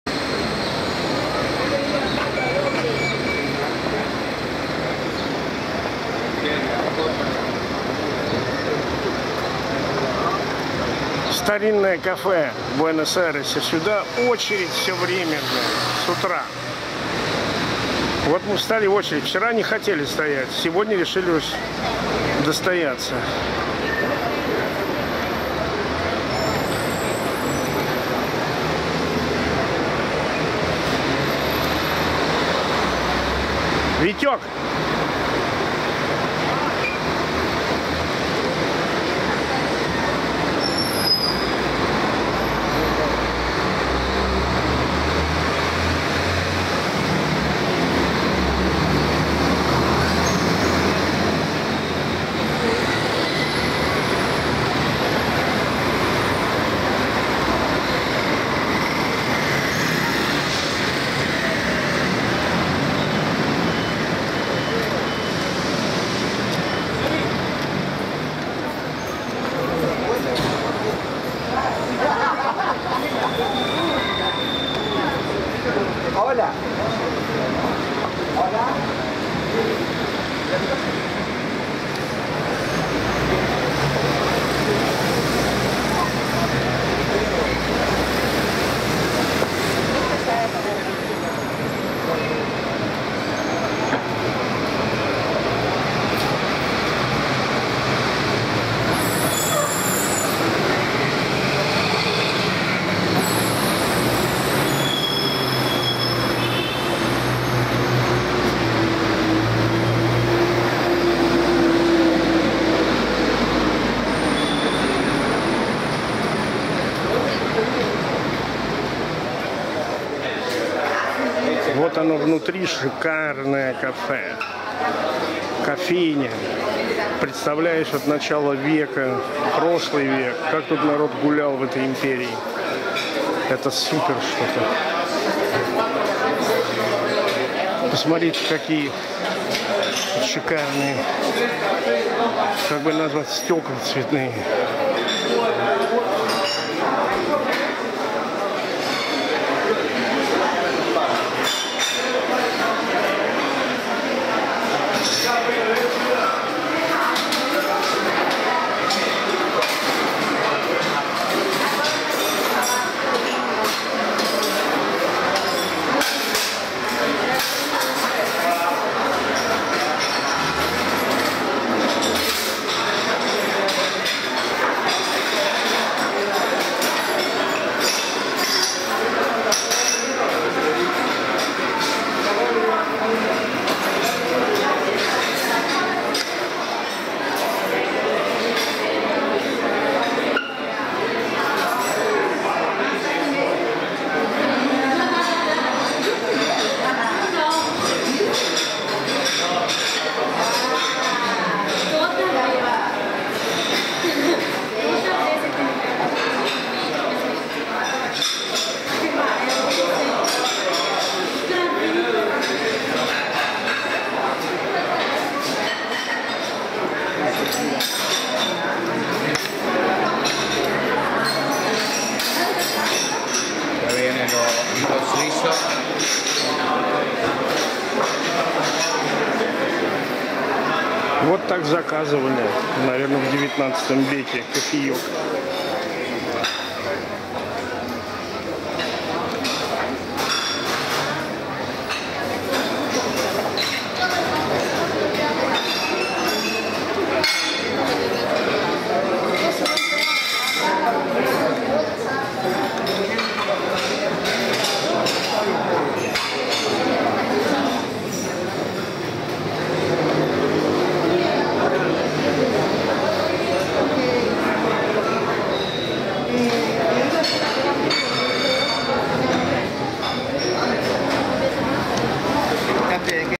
КАФЕ ТОРТОНИ - Буэнос-Айрес / CAFE TORTONI - Buenos Aires
Историческая кофейня Tortoni, в центре Буэнос - Айреса, открыта в 1858 г.  и сохранившая свой первый, исторический облик, предлагающая огромный выбор различного кофе и какао.
Съёмка моя, Gran Cafe Tortoni, Av.de Mayo 825, 23 ноября 2012
Founded in 1858, Café Tortoni is the oldest coffee shop in the whole
country. With a very rich history, it is visited at all times by intellectuals, politicians and artists...
My filming on November 23, 2012